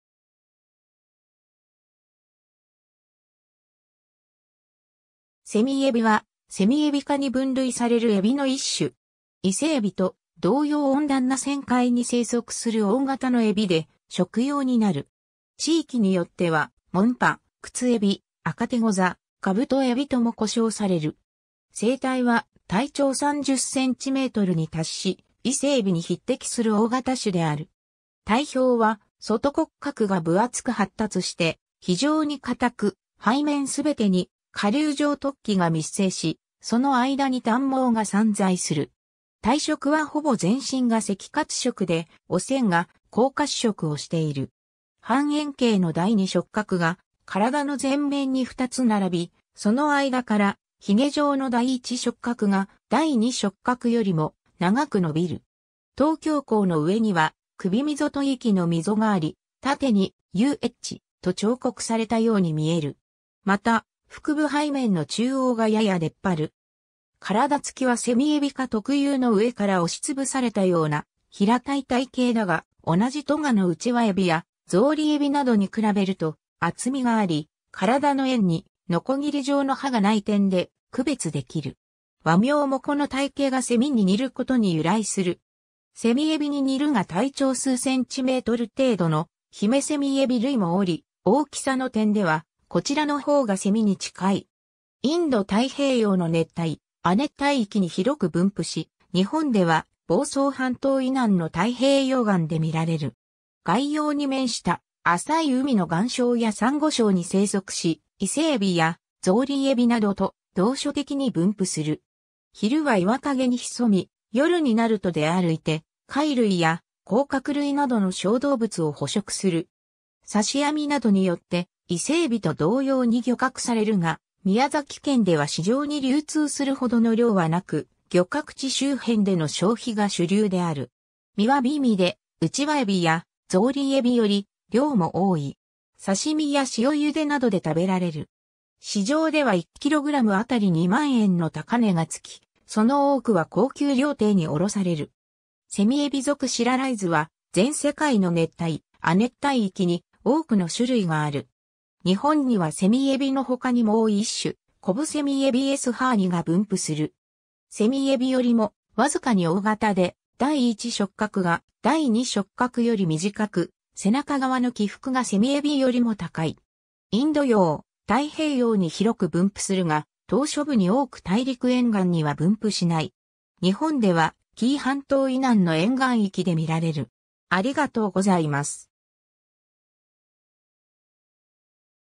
セミエビ
セミエビ（蝉海老、Scyllarides squamosus）は、セミエビ科に分類されるエビの一種。
イセエビと同様温暖な浅海に生息する大型のエビで、食用になる。
 地域によっては、モンパ、クツエビ、アカテゴザ、カブトエビとも呼称される。
 成体は体長30cmに達し、イセエビに匹敵する大型種である。
体表は外骨格が分厚く発達して非常に堅く、背面すべてに顆粒状突起が密生し、その間に短毛が散在する。
体色はほぼ全身が赤褐色で、尾扇が黄褐色をしている。
 半円形の第2触角が体の前面に2つ並び、その間からひげ状の第1触角が第2触角よりも長く伸びる。
頭胸甲の上には頸溝と胃域の溝があり、縦に"U""H"と彫刻されたように見える。
また、腹部背面の中央がやや出っ張る。
 体つきはセミエビ科特有の上から押しつぶされたような平たい体型だが、同じ科のウチワエビやゾウリエビなどに比べると厚みがあり、体の縁に鋸状の歯がない点で区別できる。
和名もこの体型がセミに似ることに由来する。
セミエビに似るが体長数cm程度のヒメセミエビ類もおり、大きさの点ではこちらの方がセミに近い。
 インド太平洋の熱帯・亜熱帯域に広く分布し、日本では房総半島以南の太平洋岸で見られる。
外洋に面した浅い海の岩礁やサンゴ礁に生息し、イセエビやゾウリエビなどと同所的に分布する。
昼は岩陰に潜み、夜になると出歩いて貝類や甲殻類などの小動物を捕食する。
 刺し網などによってイセエビと同様に漁獲されるが、宮崎県では市場に流通するほどの量はなく、漁獲地周辺での消費が主流である。
身は美味で、ウチワエビやゾウリエビより量も多い。
刺身や塩茹でなどで食べられる。
市場では1kgあたり2万円の高値がつき、その多くは高級料亭に卸される。
 セミエビ属 Scyllaridesは全世界の熱帯・亜熱帯域に多くの種類がある。
 日本にはセミエビの他にもう一種コブセミエビ（瘤蝉海老） S. haani (De Haan, 1841) が分布する。
セミエビよりもわずかに大型で、第1触角が第2触角より短く、背中側の起伏がセミエビよりも高い。
インド洋・太平洋に広く分布するが、島嶼部に多く大陸沿岸には分布しない。
日本では紀伊半島以南の沿岸域で見られる。